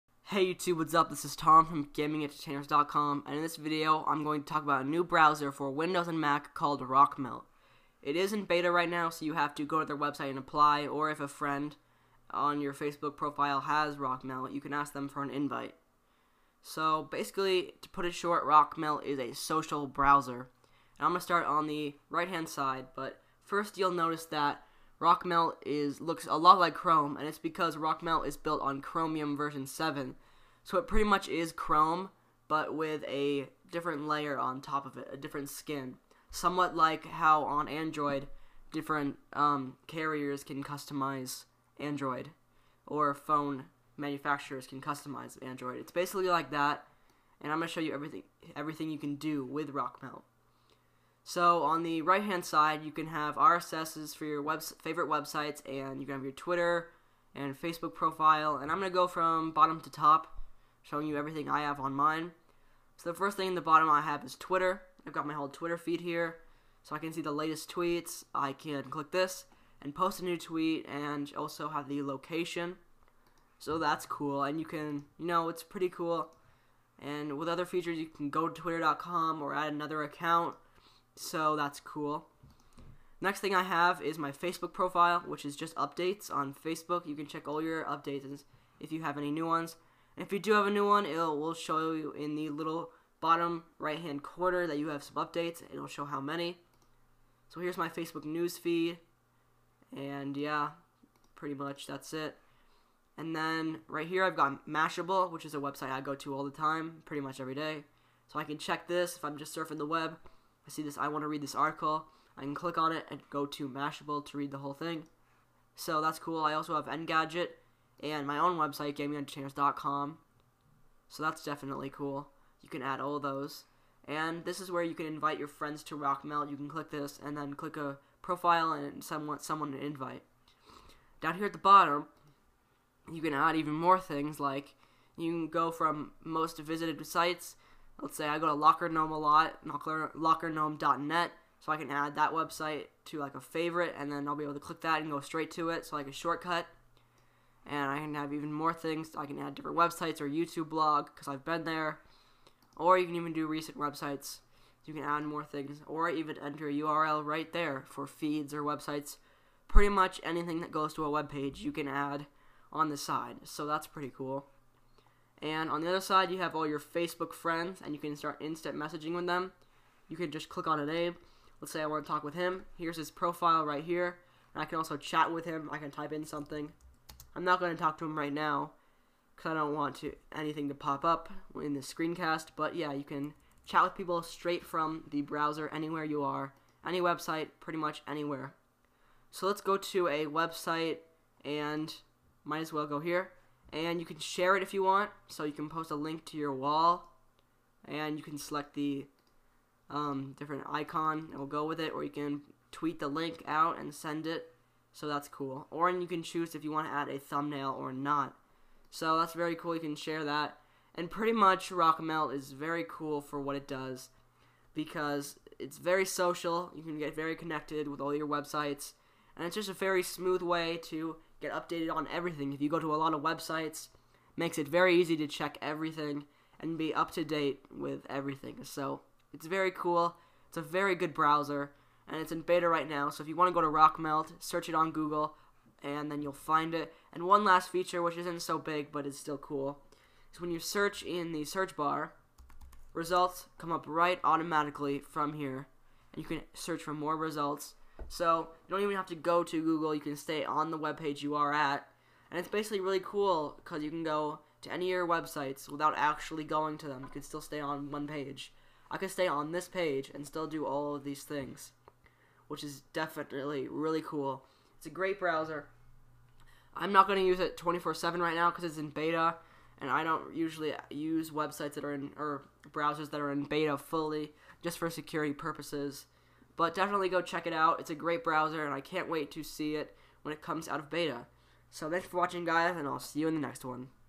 RockMelt - The New Social Browser
In this video I demo the beta version of a new internet browser called "RockMelt". RockMelt is unique because it connects you to all your favorite social networks straight from the browser with no need to go to the website. RockMelt is currently in beta but you can request an invite here: